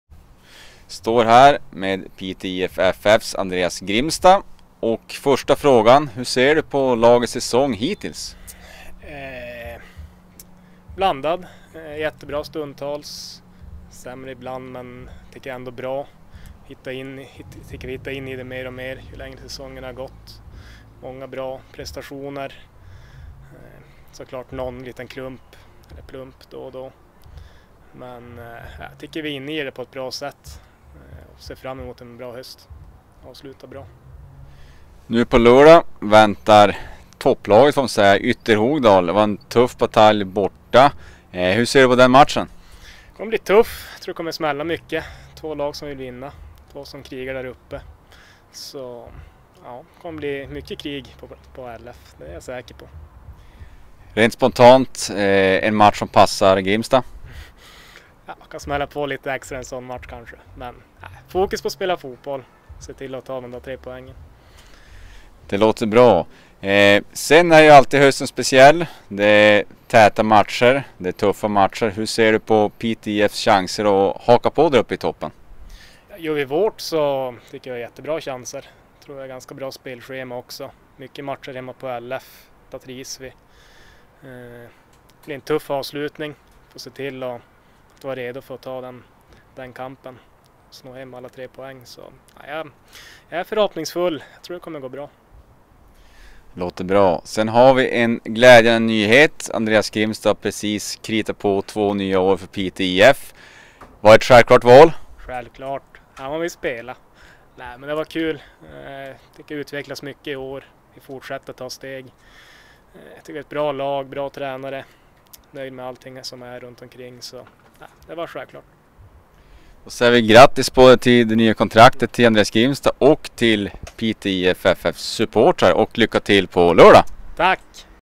Andreas Grimstad inför Ytterhogdal
Andreas Grimstad pratar om:
- Nya kontraktet
- Tankar inför matchen mot Ytterhogdal
- Lagets prestation m.m